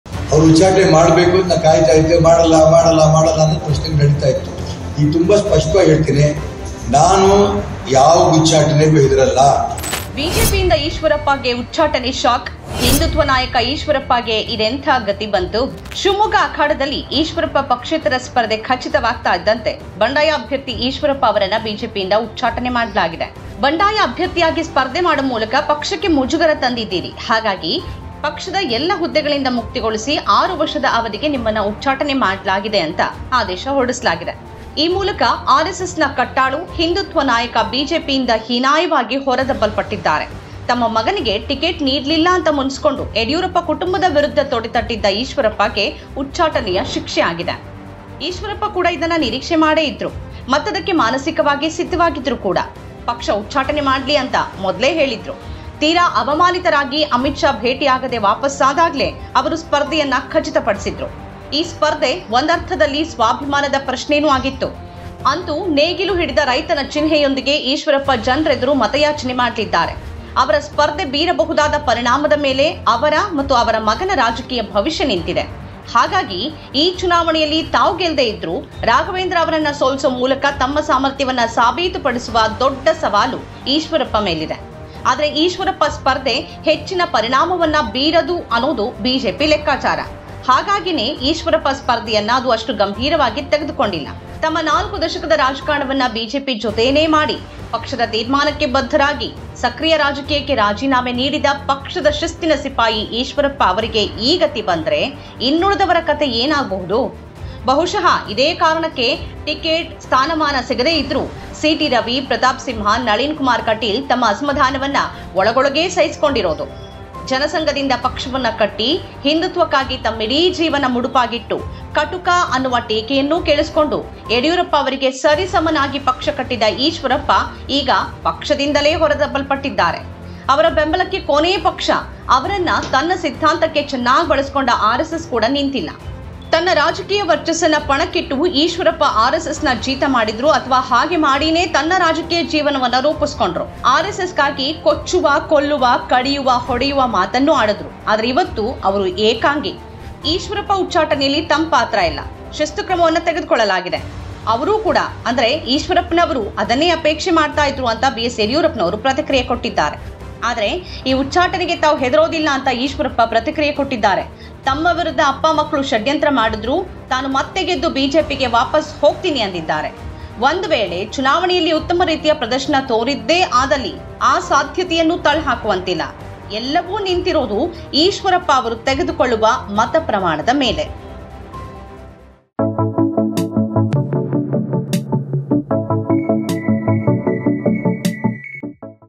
ಬಿಜೆಪಿಯಿಂದ ಈಶ್ವರಪ್ಪಗೆ ಉಚ್ಚಾಟನೆ ಶಾಕ್ | BJP | K. S. Eshwarappa | RSS
ಜೀವನವಿಡೀ ಆರೆಸ್ಸೆಸ್ ಜೀತ ಮಾಡಿದ  ಈಶ್ವರಪ್ಪ ಈಗ ಏಕಾಂಗಿ